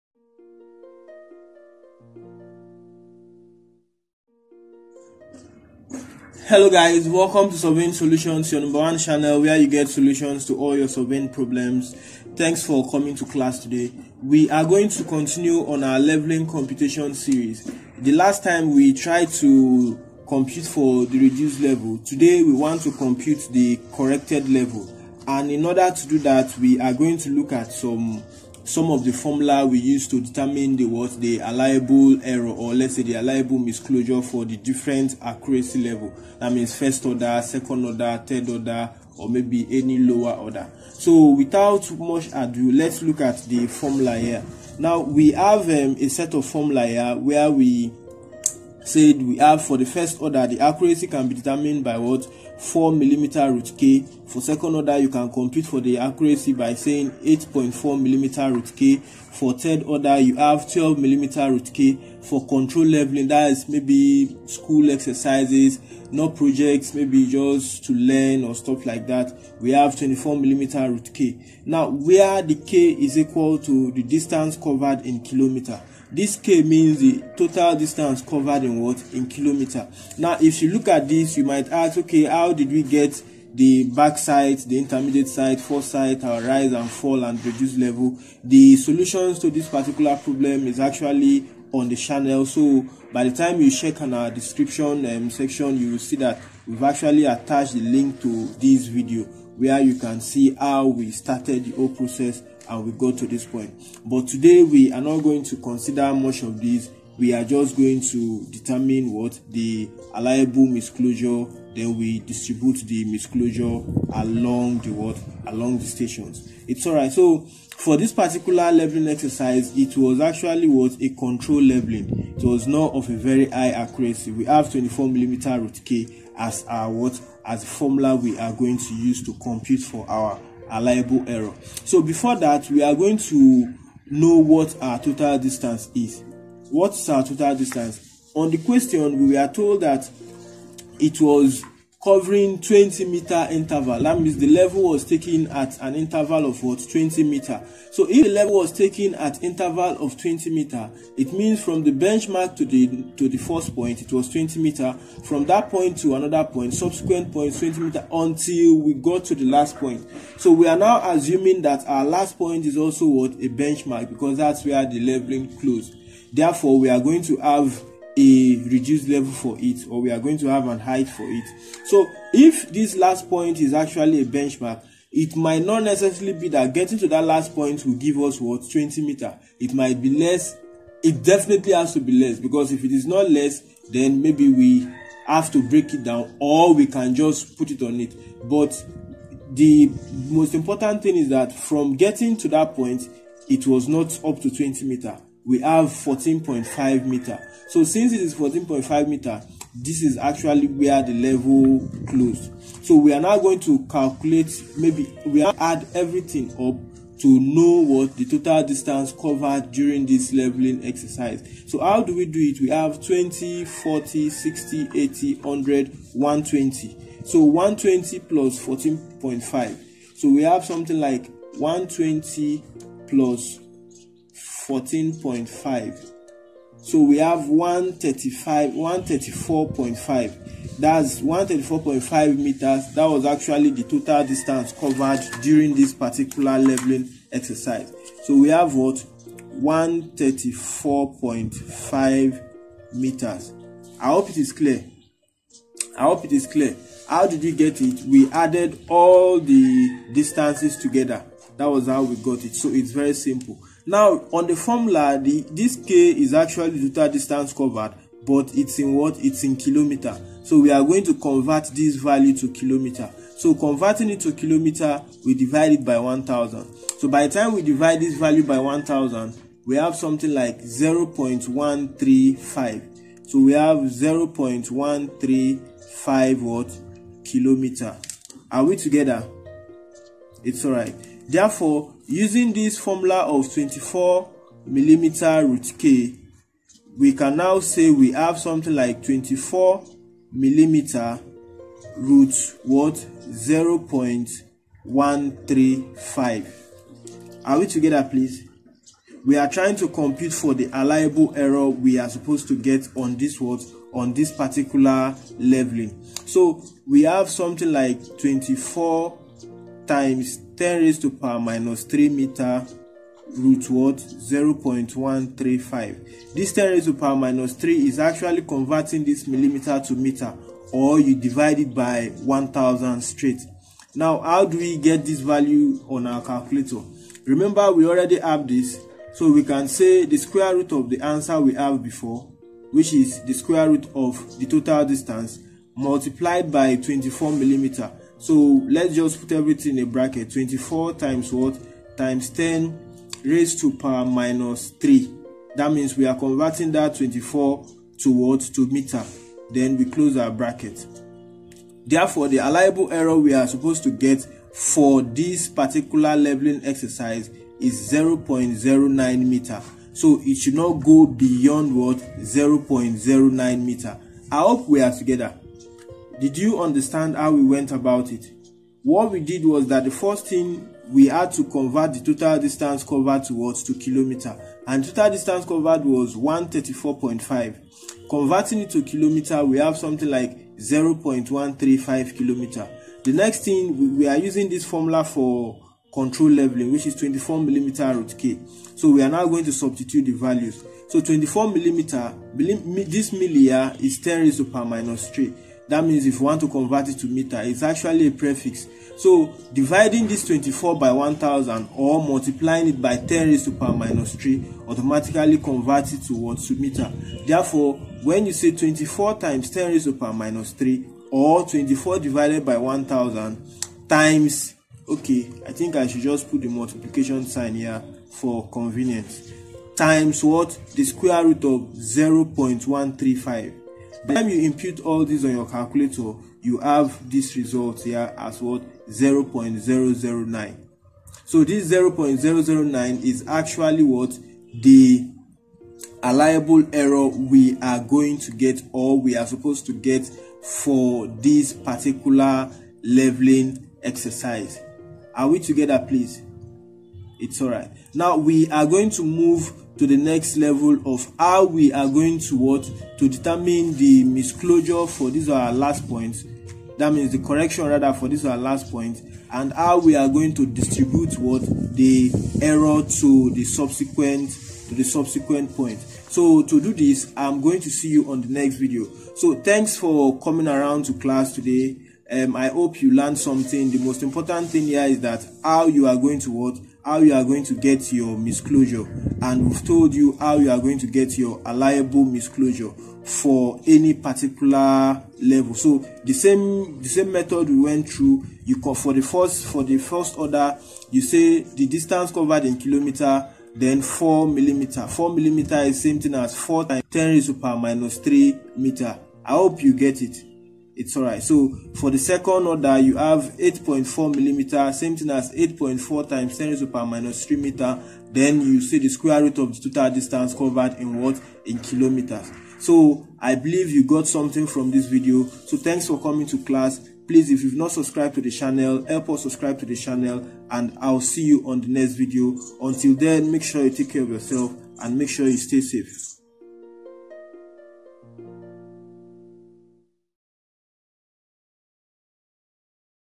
Determination of Levelling Misclosure | Levelling Computation 2025
Levelling is the process of determining the change in elevation between points. It is a is a process of determining the height of one level relative to another.

It is used in surveying to establish the elevation of a point relative to a datum, or to establish a point at a given elevation relative to a datum.

Height of Collimation Method
It consists of determining the elevation of the plane of collimation (H.I.) for every set up of the instrument and to obtain the reduced level of point to with reference to the respective plane of collimation.
In other words, we determine the reduced level of points by making reference to the height of instrument set up.

Rise and Fall Method
It consists of determining the difference of elevation between consecutive points by comparing each point after the first that immediately preceding it in the direction of travel.

Levelling Misclosure
On this video we explained how to determine the allowable error (misclosure) in any levelling exercise. This can be achieved by using the different formulae for the different order of accuracy.